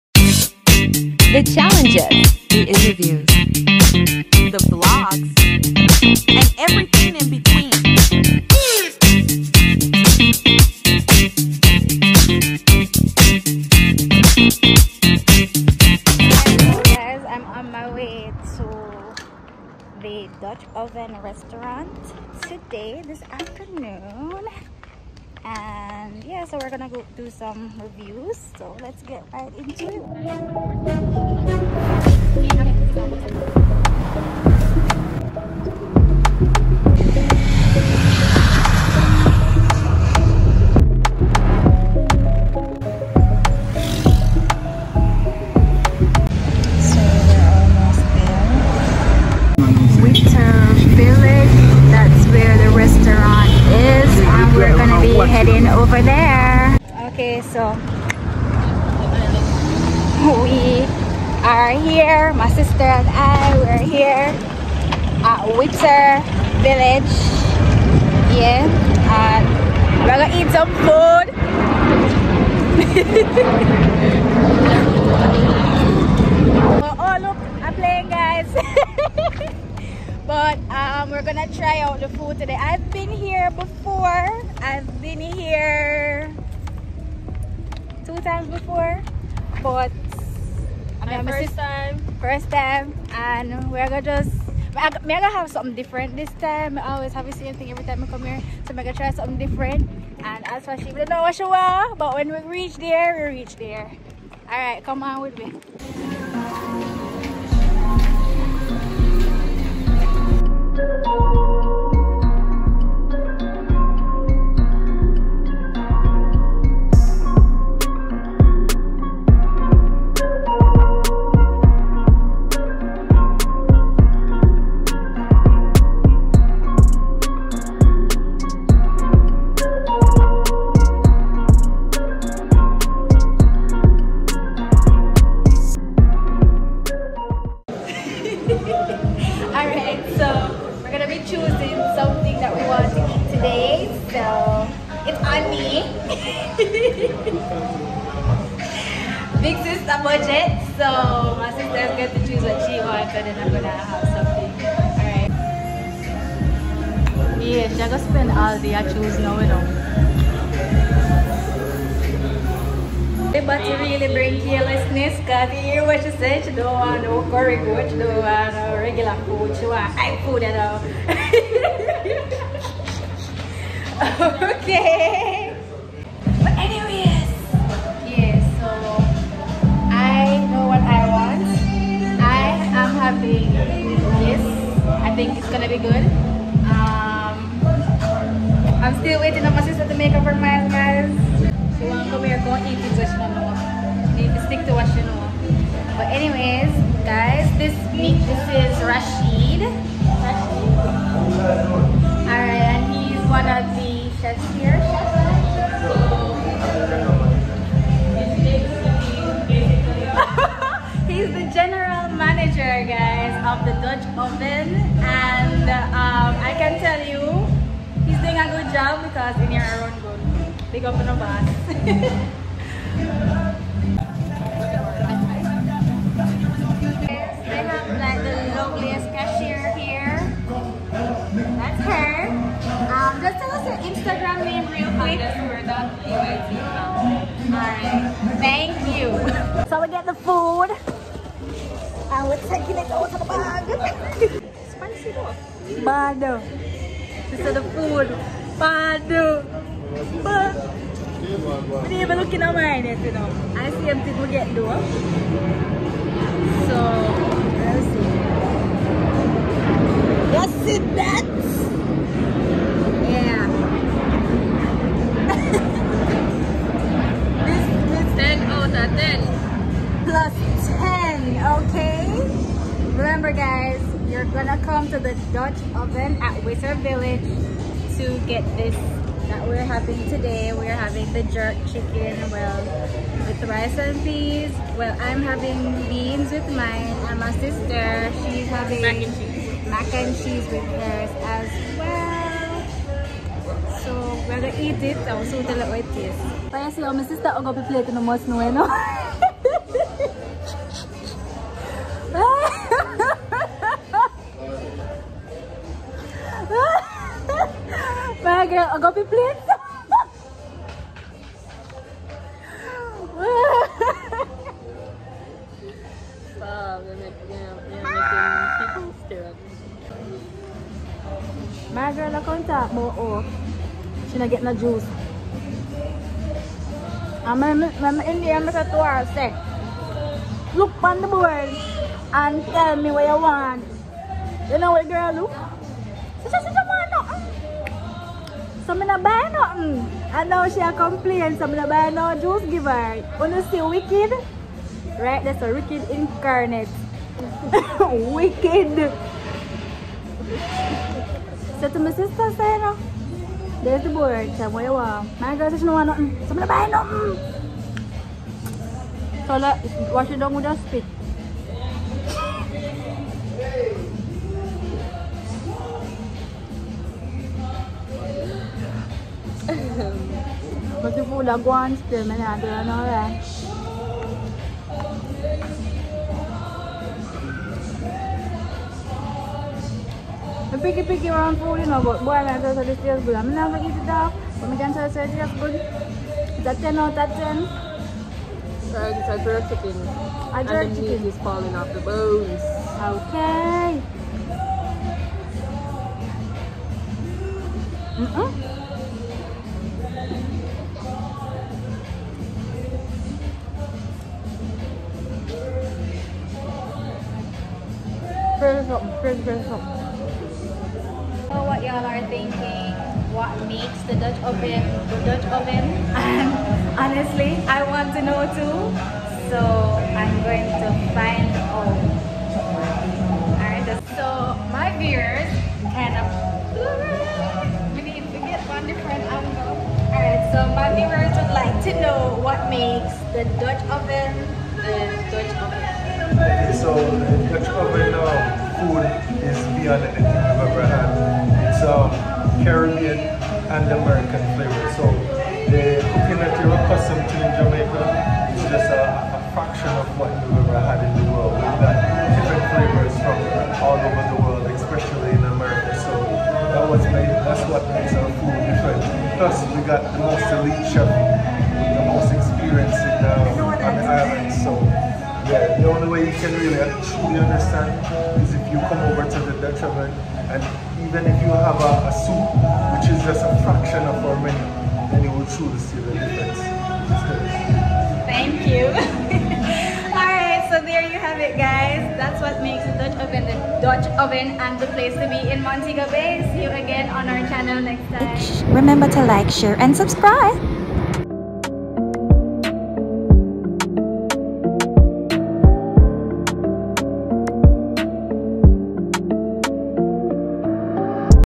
The Dutch oven Restaurant review vlog.
My sister and I went out to the Dutch oven in Montego Bay, Ironshore, St. James to try their food and find out what makes the Dutch oven one of the places to be in Jamaica.